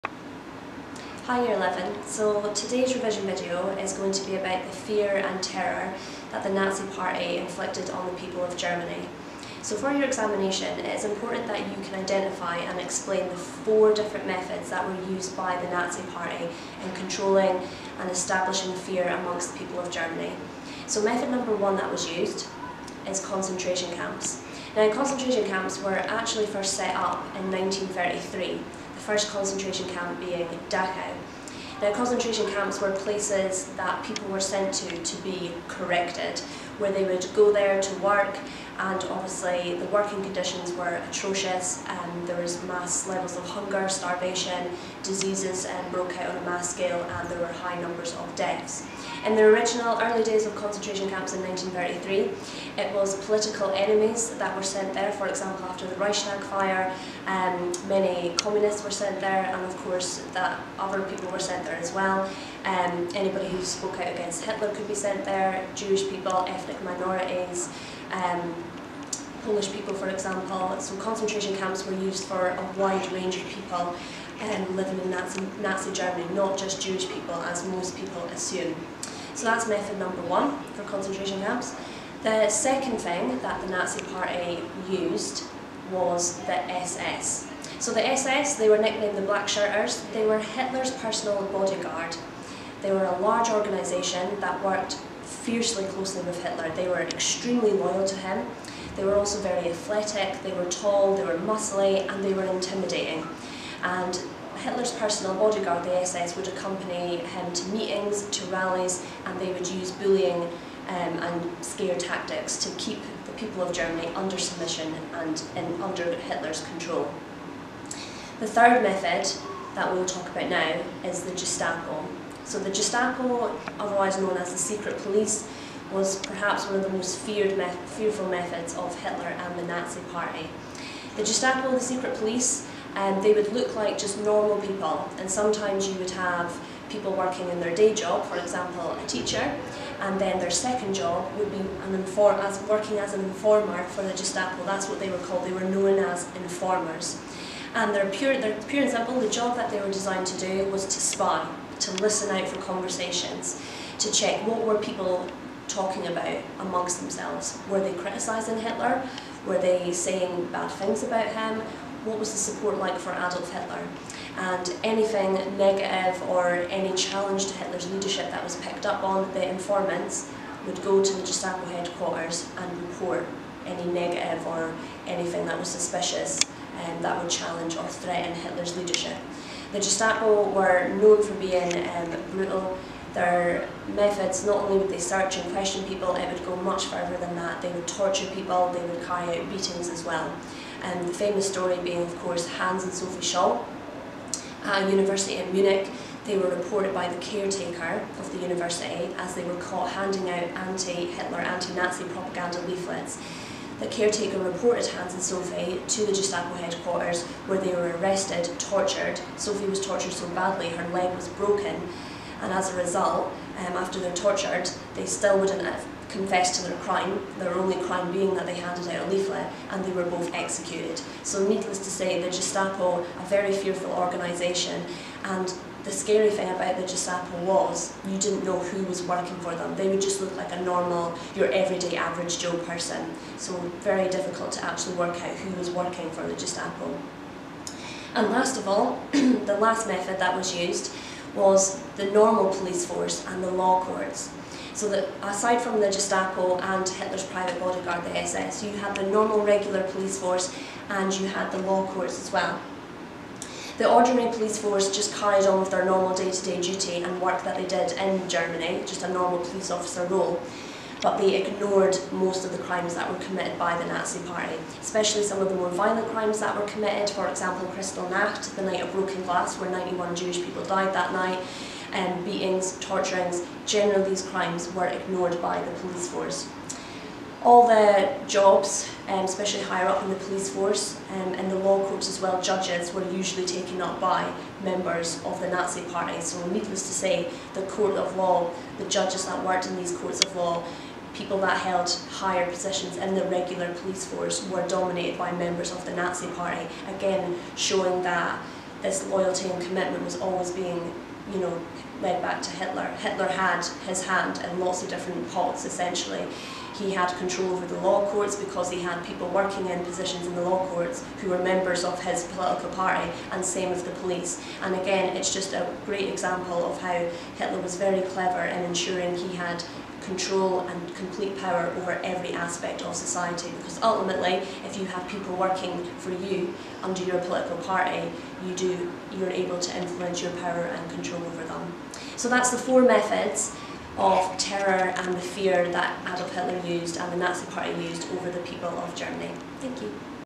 Video 10 AQA GCSE History: Paper 1 Germany 1890-1945 TERROR in Nazi Germany
This video goes through key information about Terror in Nazi Germany between the years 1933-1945.